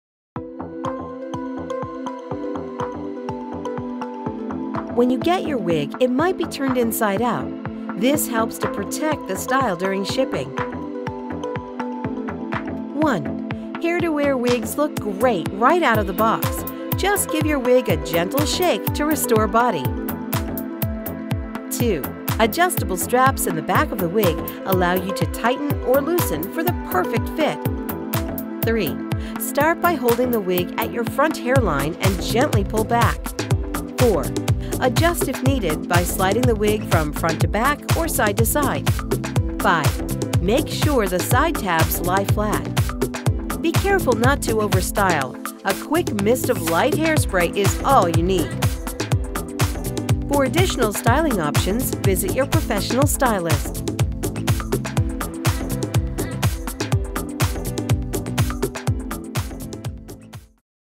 How To Put on Your Wig | Christie Brinkley Wig Collection
See how quick and easy it is to put on your Christie Brinkley wig in just a few simple steps. All Christie Brinkley wigs are ready-to-wear!